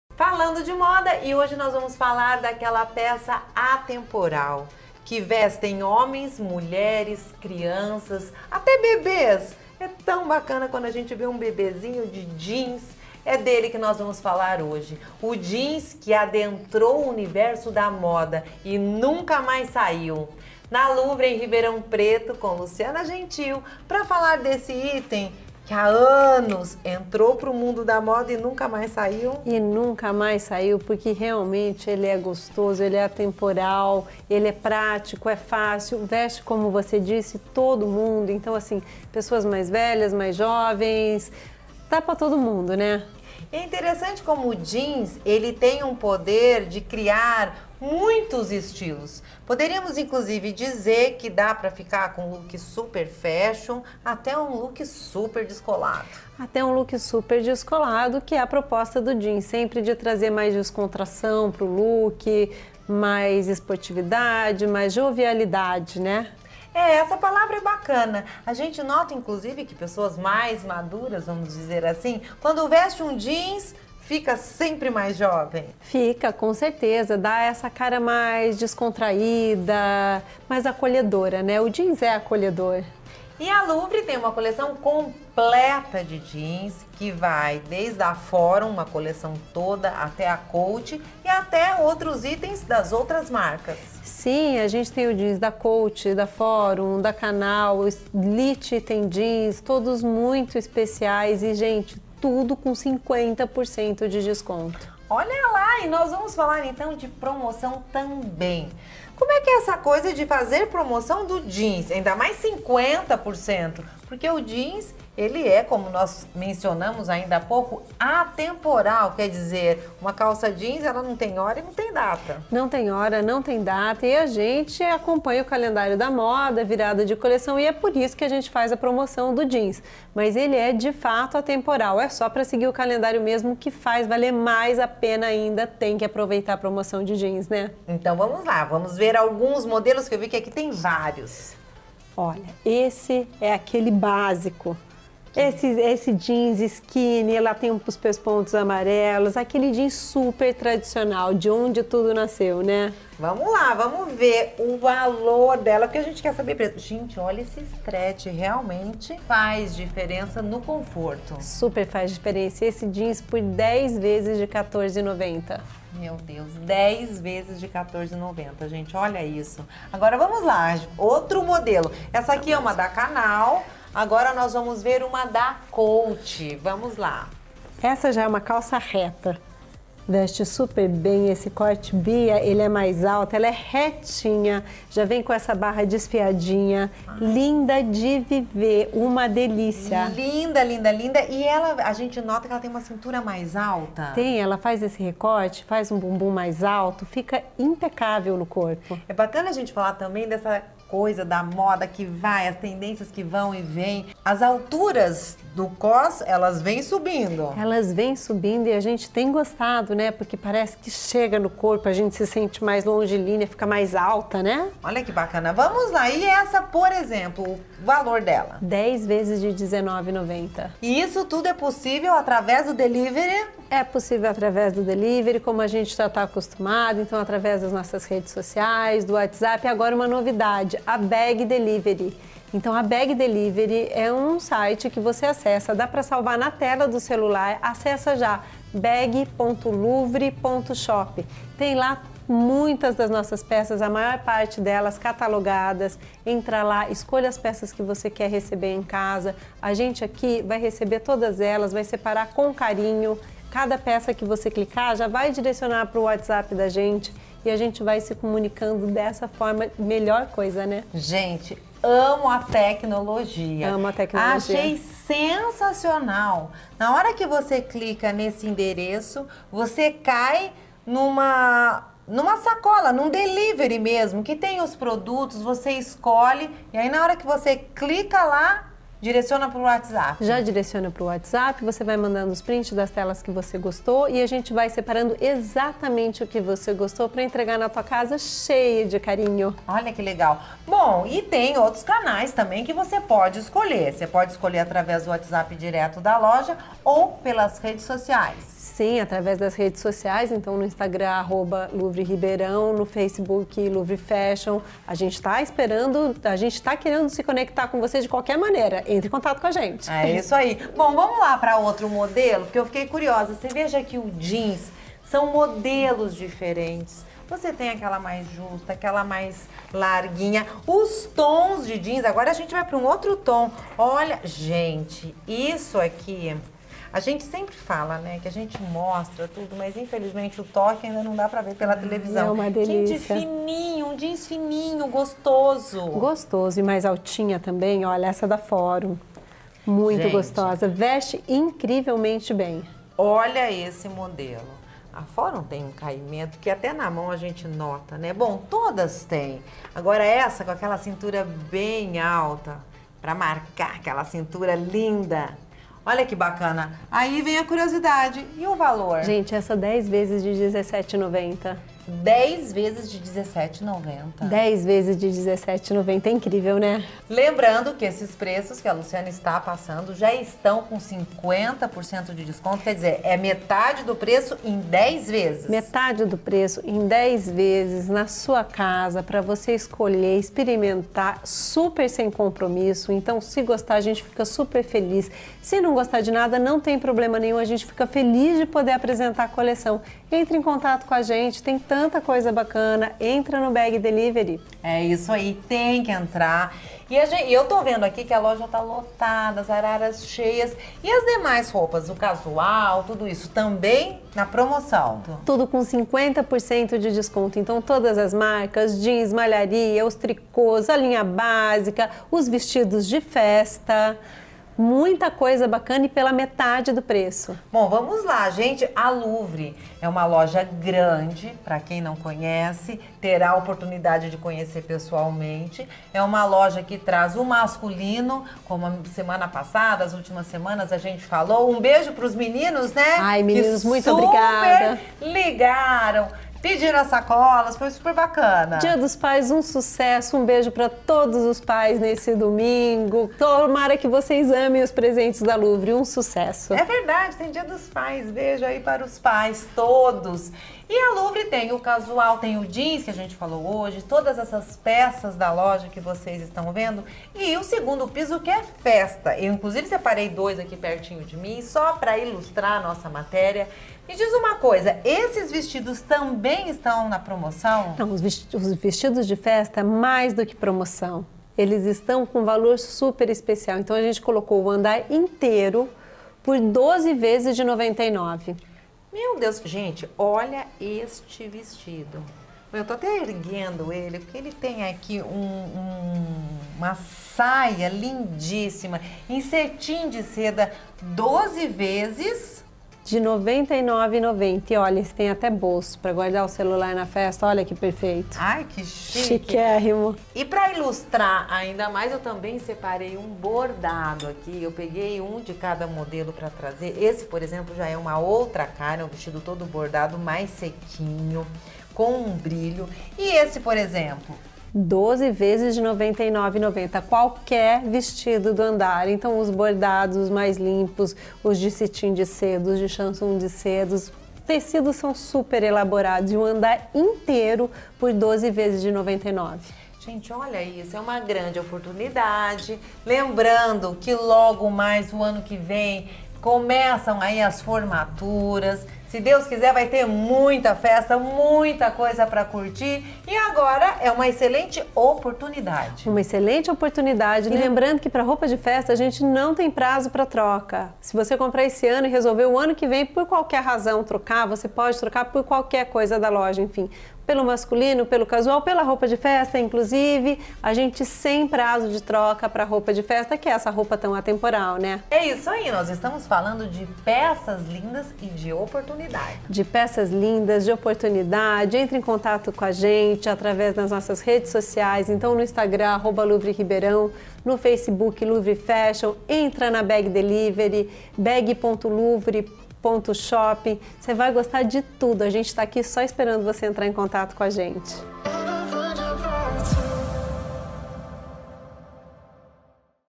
DICAS DE PEÇAS QUE NUNCA SAEM DE MODA | LELINHA GENTIL
Luciana Gentil, da Louvre Ribeirão Preto traz dicas de peças atemporais, que nunca saem de moda. Confira!